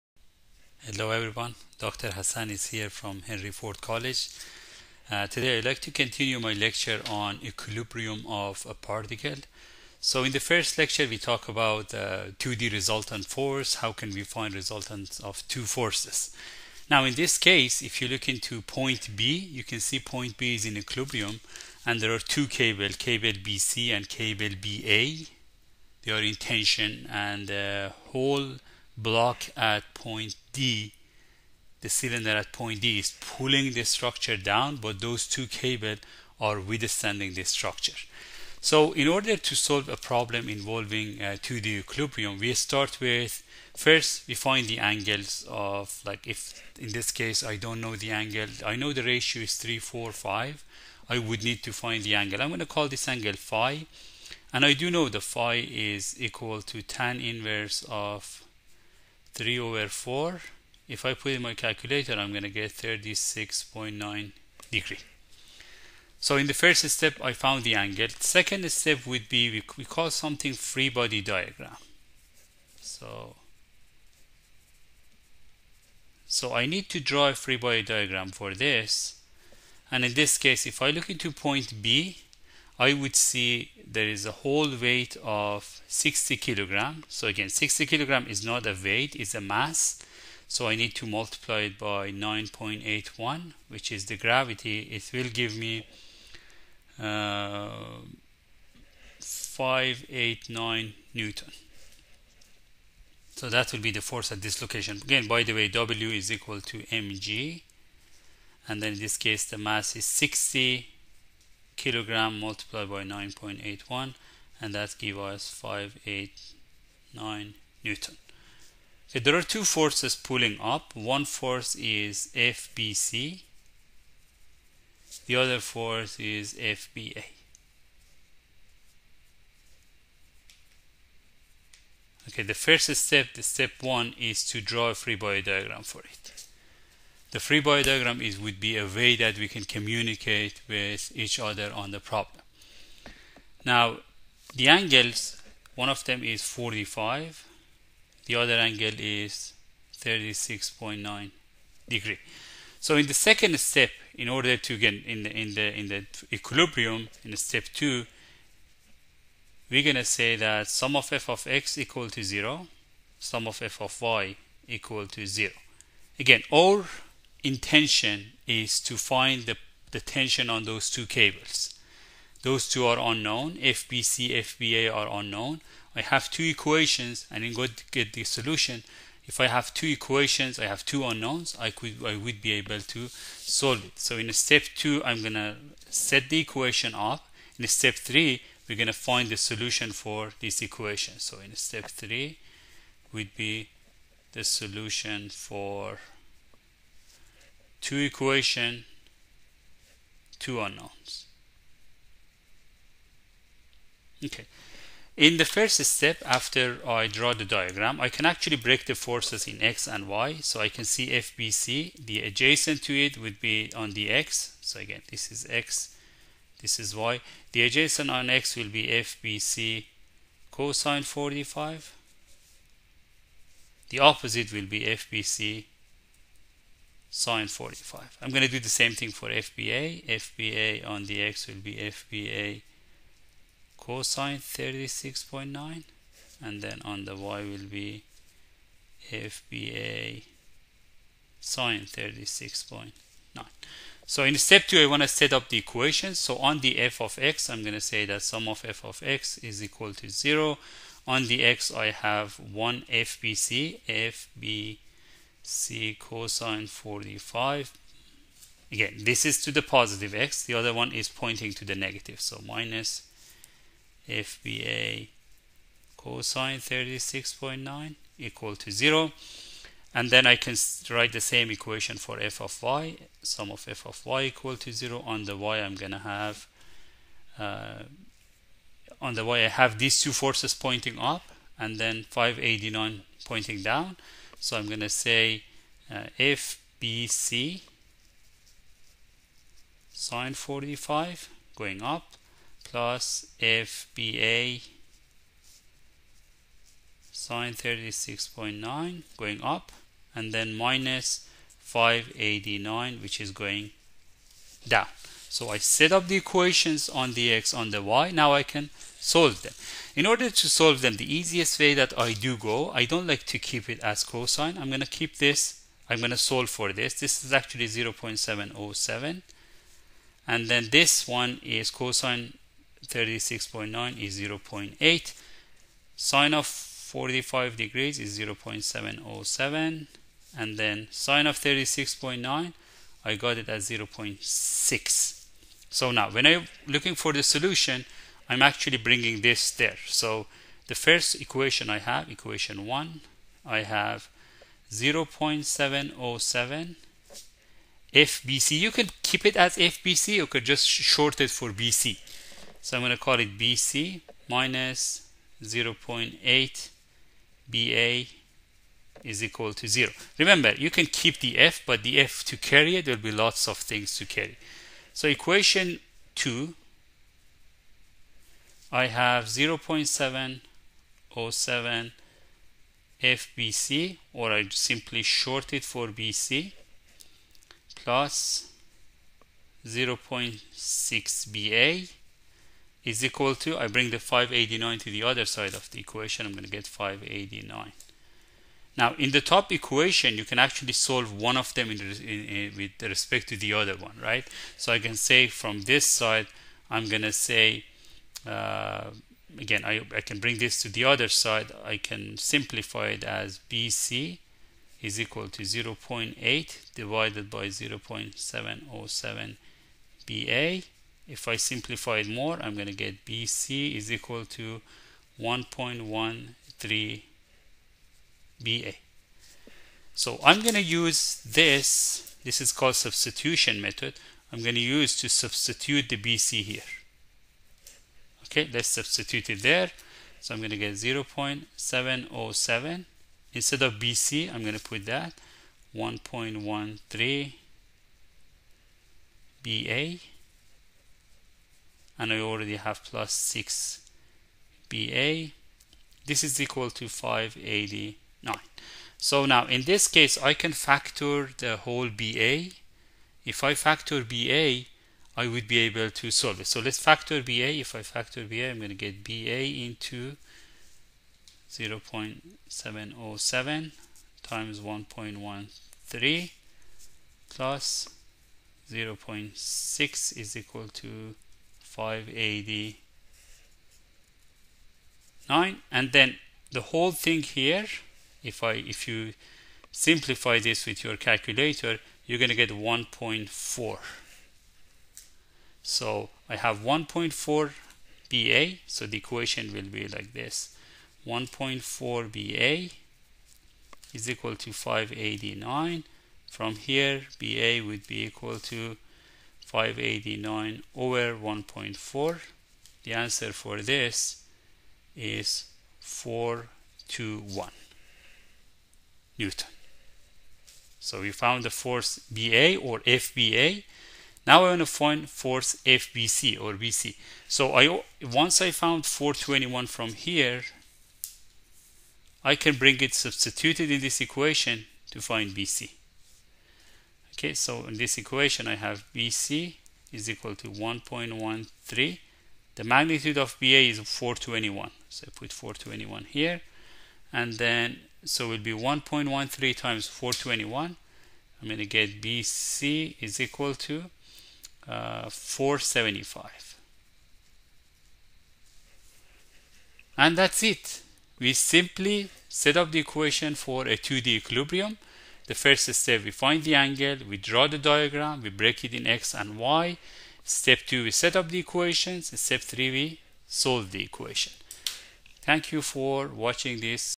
Lecture 3: 2D Equilbrium of Particle
Determine the tension in cables BA and BC necessary to support the
60-kg cylinder in fig. 3—6a. A Question from Engineering Mechanics: Statics.